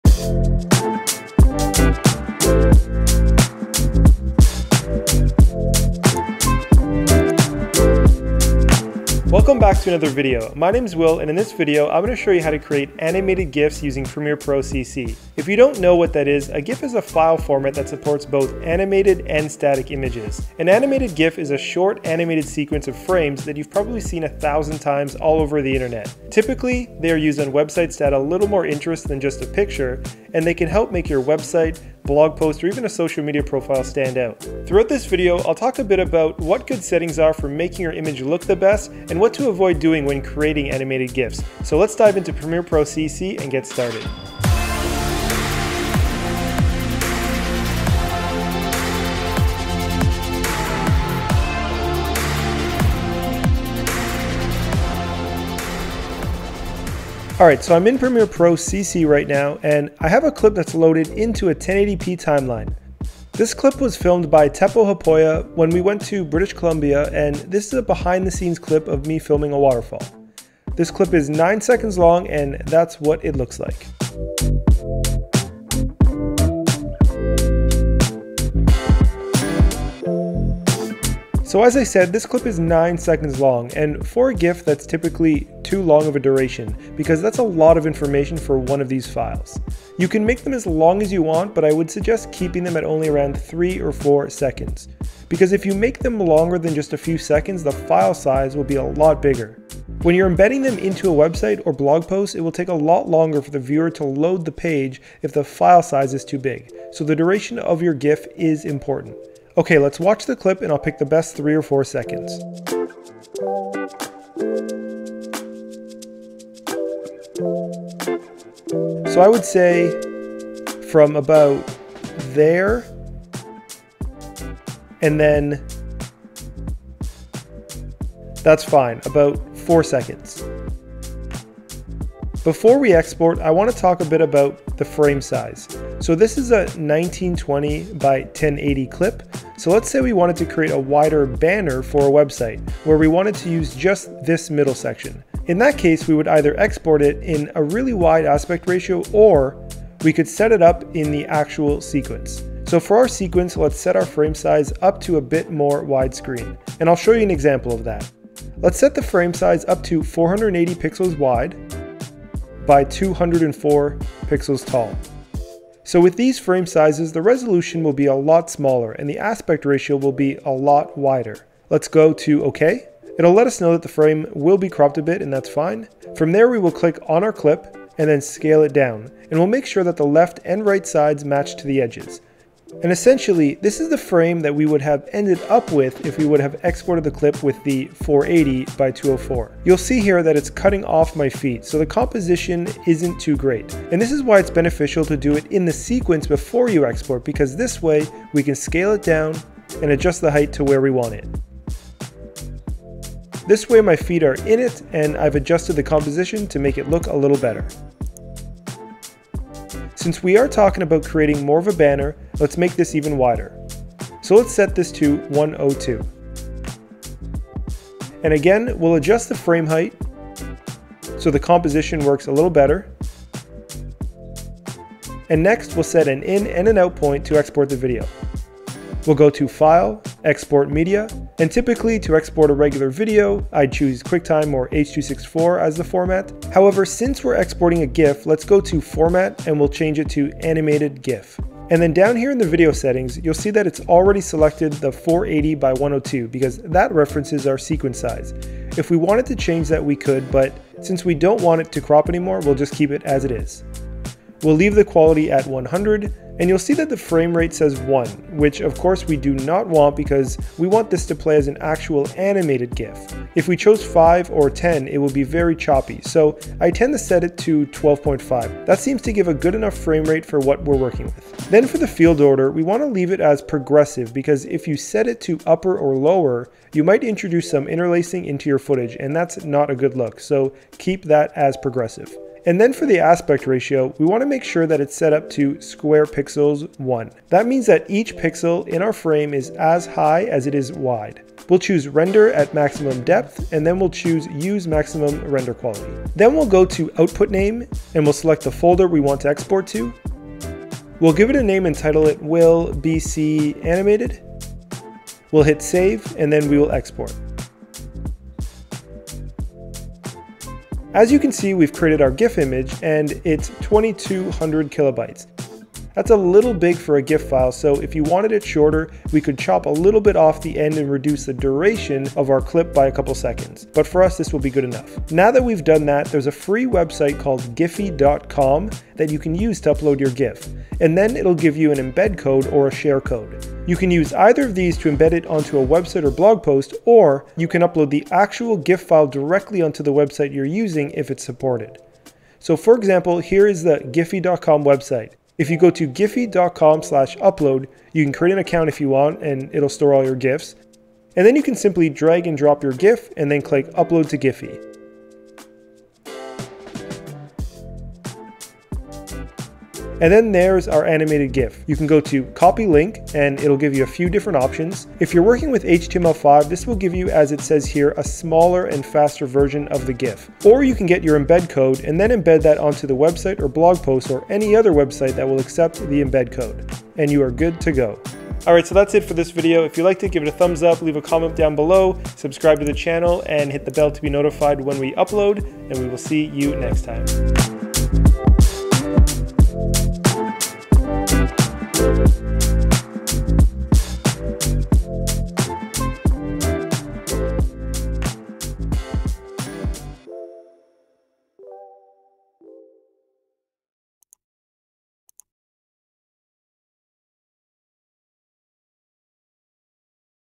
How To Create ANIMATED GIFs In Premiere Pro CC
Learn how to create a Animated GIFs inside Adobe Premiere Pro CC with this video tutorial from Will.

If you don't have Premiere Pro, you can download a 30 day trial off of Adobe's website. Or to create a Gif from a video without using Premiere Pro; Google search 'convert video to gif online' and there will be several website search results that should be able to do the trick :)